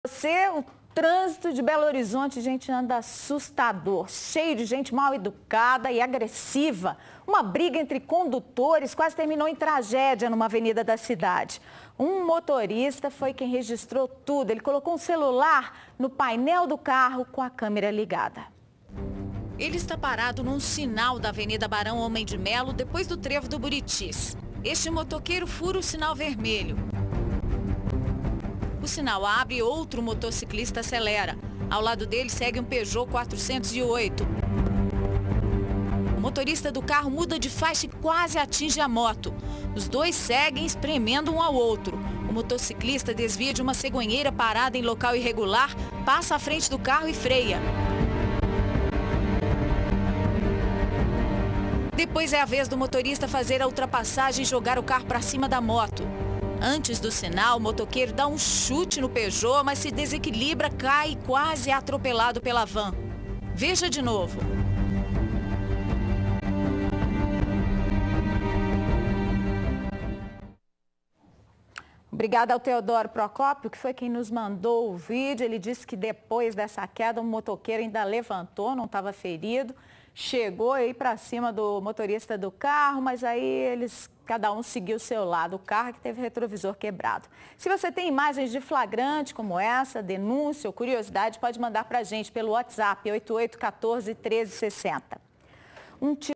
Motoqueiro e motorista se desentendem no trânsito e briga quase termina em tragédia; veja
Uma briga de trânsito quase terminou em tragédia na Avenida Barão Homem de Melo, em Belo Horizonte. O motorista de um carro e um motoqueiro se desentenderam e o motociclista chutou o carro em movimento. Com o impacto, ele se desequilibrou e caiu. Por pouco não foi atingido por um carro, que vinha na outra pista.